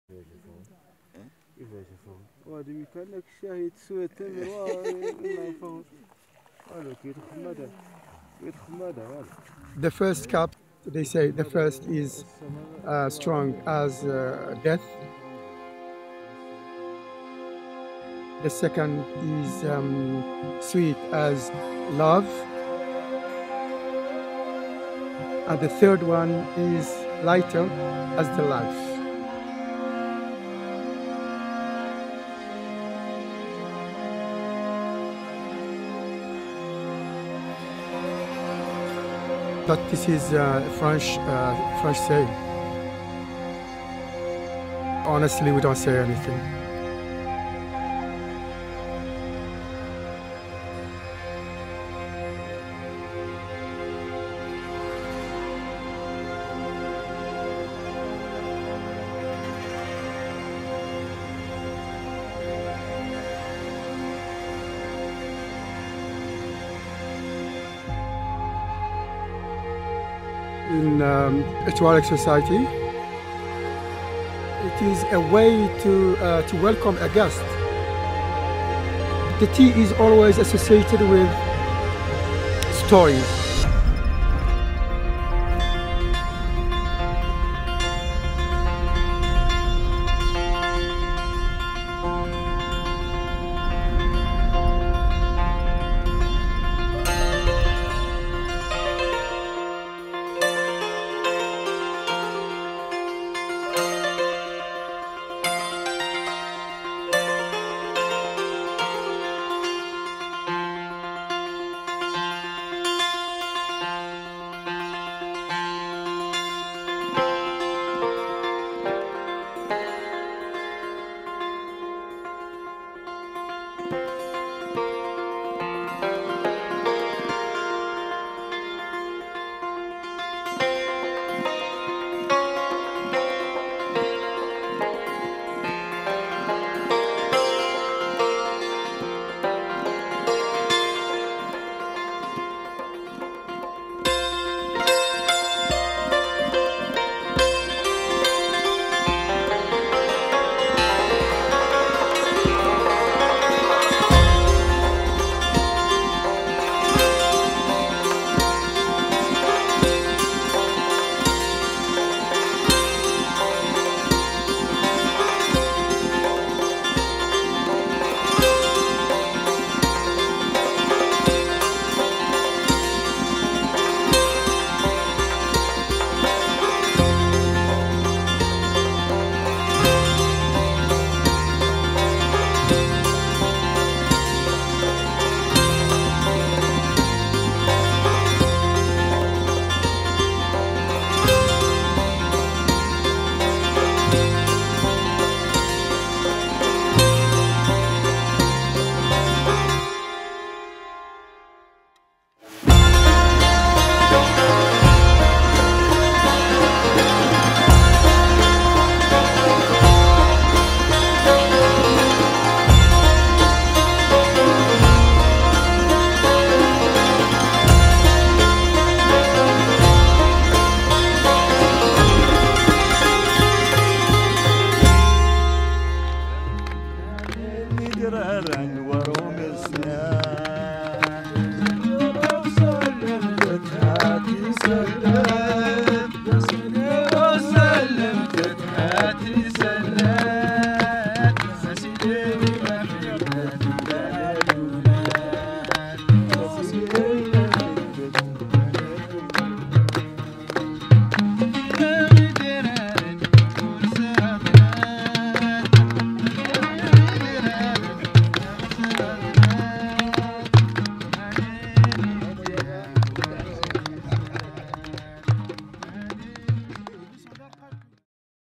Tea with the Tuaregs | The Algerian Sahara Desert
Camping and exploring in the Algerian Sahara Desert with the Tuareg people, February 2019.

Shot with:
Canon 5D Mark IV
Canon 24-70mm 2.8 II
Canon 70-200mm 2.8 II
Canon 50mm 1.4
DJI Ronin-S